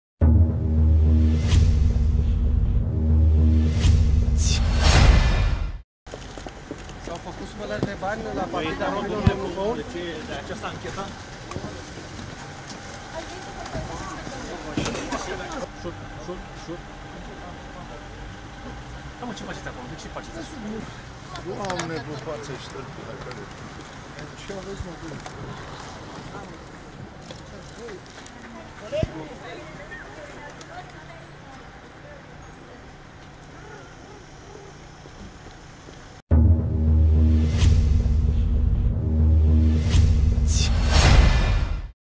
Deputatul Nicolae Păun, la DNA
Deputatul Nicolae Păun s-a prezentat miercuri la Direcția Națională Anticorupție, fiind citat de procurori.